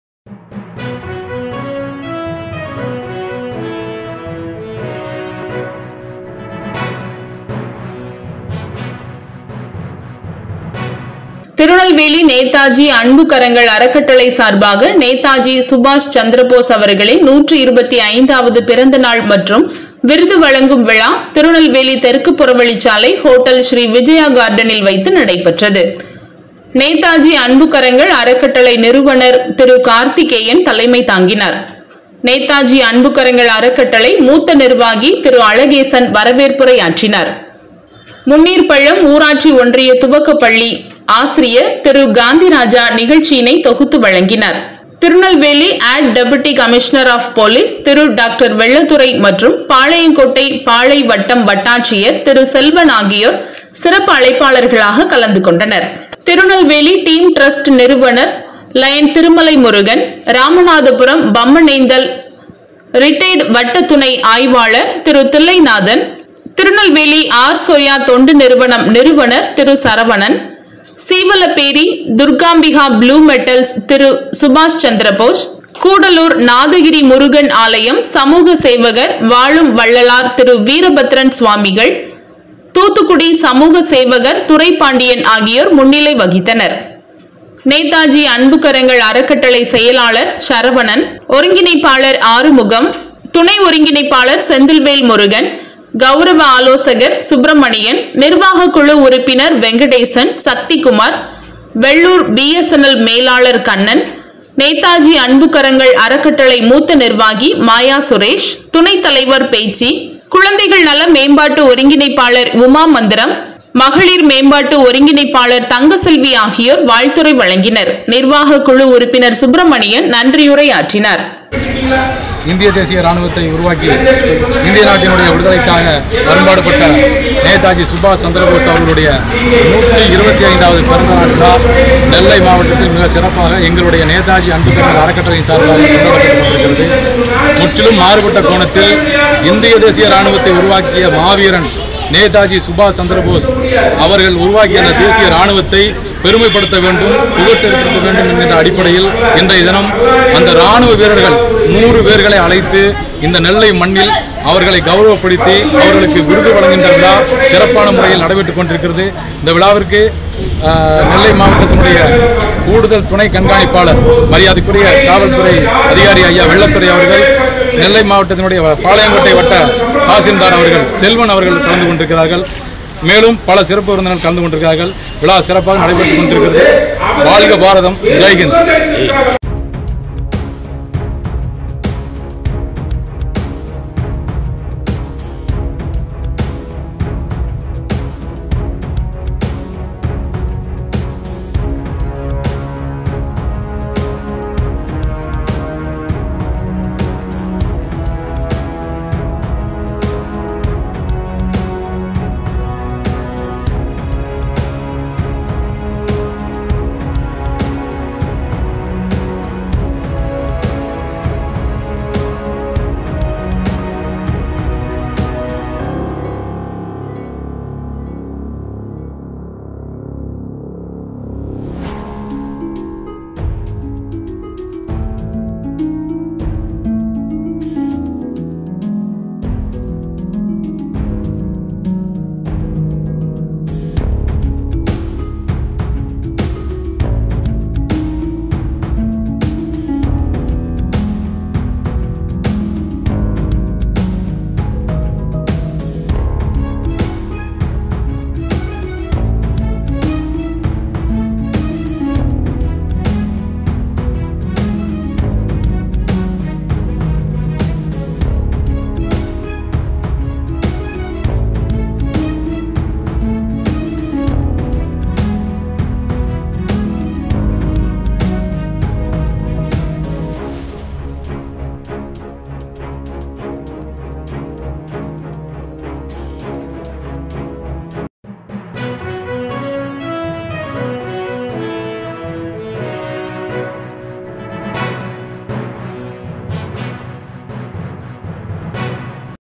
ஹோட்டல் ஸ்ரீ விஜயா கார்டனில் நேதாஜி சுபாஷ் சந்திரபோஸ் பிறந்தநாள் விழா மற்றும் விருது வழங்கும் விழா
திருநெல்வேலி ஹோட்டல் ஸ்ரீ விஜயா கார்டனில் நேதாஜி அன்புக்கரங்கள் அறக்கட்டளை சார்பில் நேதாஜி சுபாஷ் சந்திரபோஸ் அவர்களின் 125வது பிறந்தநாள் விழா மற்றும் விருது வழங்கும் விழா நடைபெற்றது